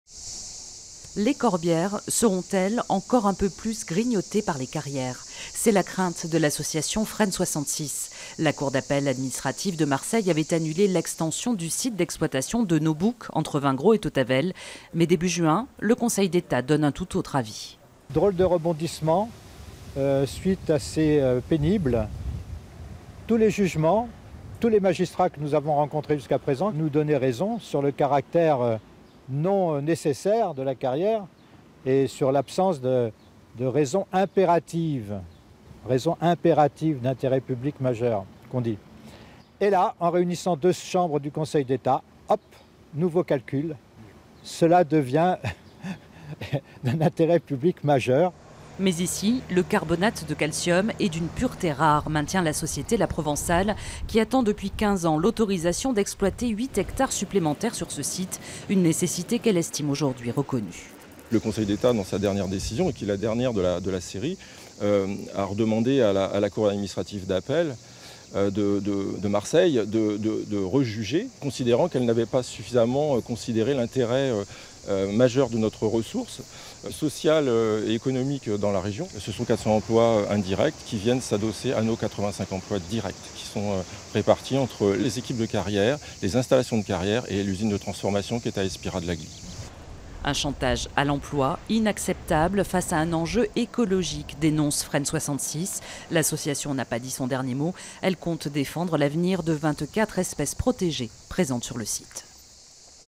Carrière de Vingrau : le Conseil d'Etat juge l'extension d'un "intérêt public majeur"
Rebondissement dans le feuilleton judiciaire qui oppose les détracteurs du projet d'extension de la carrière de marbre blanc de Nau-Bouques, entre Vingrau et Tautavel (Pyrénées-Orientales) à l'exploitant du site : le Conseil d'Etat vient de donner raison à ce dernier. Explications dans ce reportage d'Aude Cheron et Céline Llambrich, avec Marc Maillet, président de l'association environnementale Frene 66 et Philippe Quero, directeur usine La Provençale.